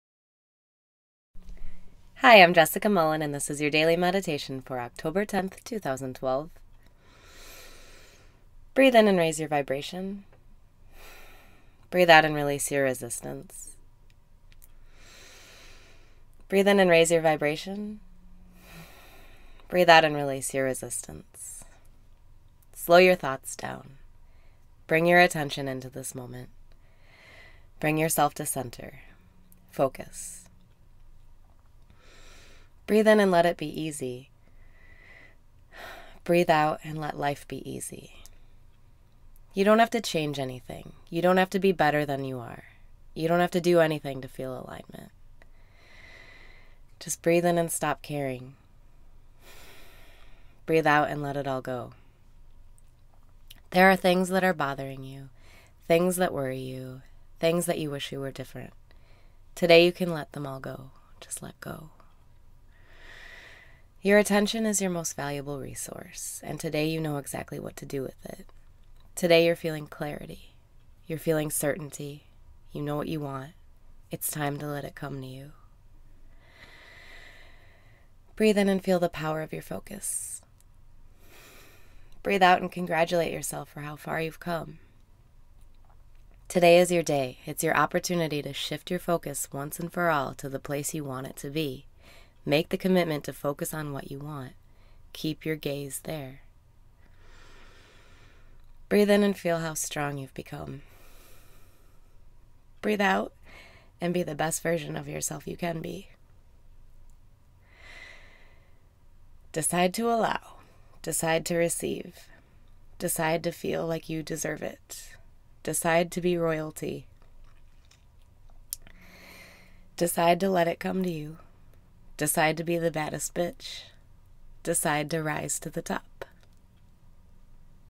Daily Meditation for October 10, 2012 - Focus
Slow your thoughts down. Bring your attention into this moment. Bring yourself to center. Focus.

Breathe in and let it be easy.
Breathe out and let life be easy.

You don't have to change anything. You don't have to be better than you are. You don't have to do anything to feel alignment.

Breathe in and stop caring.
Breathe out and let it all go.

There are things that are bothering you. There are things that worry you. There are things that you wish were different. Today, you can let them all go. Just let go.

Your attention is your most valuable resource. Today you know exactly what to do with it. Today you are feeling clarity. You are feeling certainty. You know what you want. It's time to let it come to you.

Breathe in and feel the power of your focus.
Breathe out and congratulate yourself for how far you've come.

Today is your day. It is your opportunity to shift your focus once and for all, to the place you want it to be. Make the commitment to focus on what you want. Keep your gaze there.

Breathe in and feel how strong you've become.
Breathe out and be the best version of yourself you can be.

Decide to allow. Decide to receive. Decide to feel like you deserve it. Decide to be royalty. Decide to let it all come to you. Decide to be the baddest bitch. Decide to rise to the top.